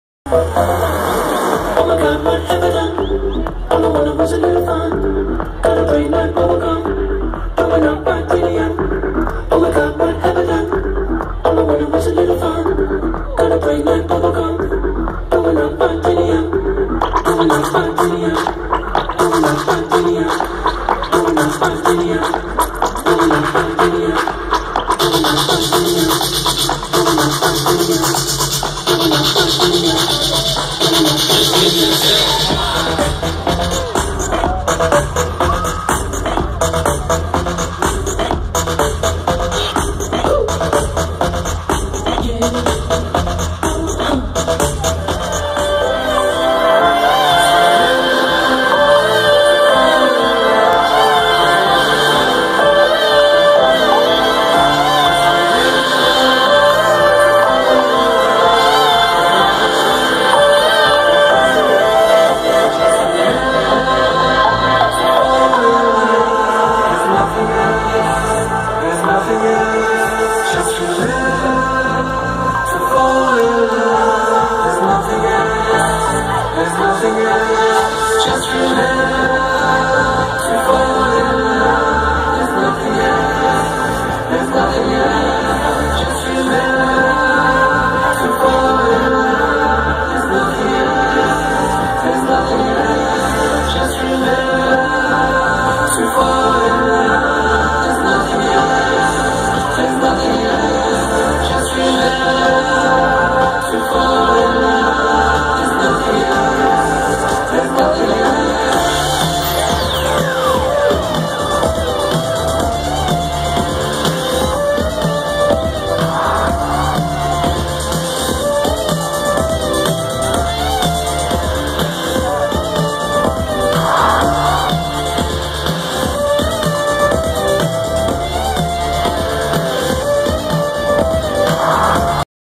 The Chemical Brothers - Live @ HARD Summer 2015
Day 1 - HARDer Stage

Closing out with the guys that started it all for me. Absolute Legends!!